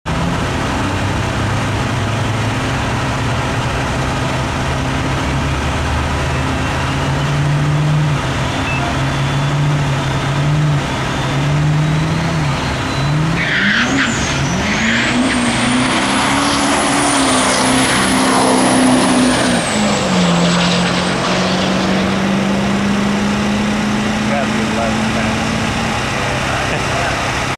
Whiteout 11.56 @ 126.MPG
Ken's Whiteout with an 11.56 at the fri night test n tune in pheonix